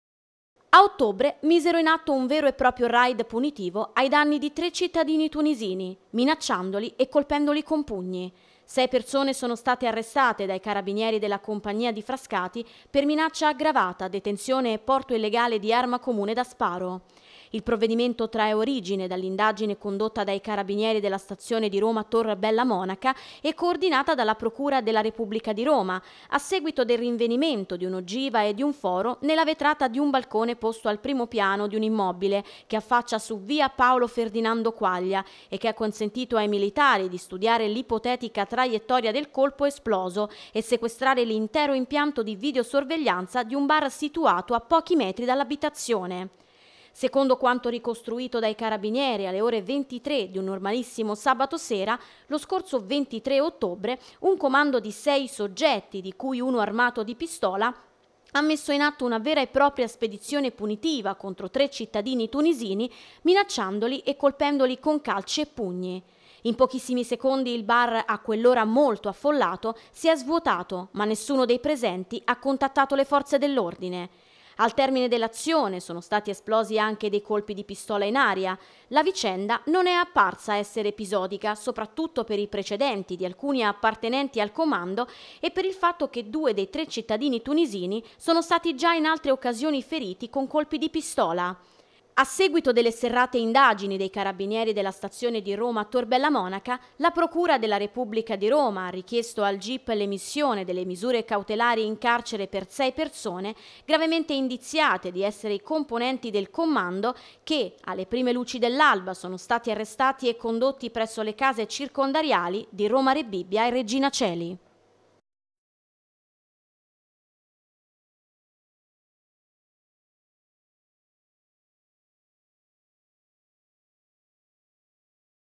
28.12.2022 ROMA: SPEDIZIONE PUNITIVA, 6 ARRESTI NEL CLAN MOCCIA
Spedizione punitiva a Tor Bella Monaca, sei arrestati. Sono Gaetano Moccia, Pasquale Moccia, Denny Moccia, Emanuele Selva e i fratelli Cristian e Danilo Rosati.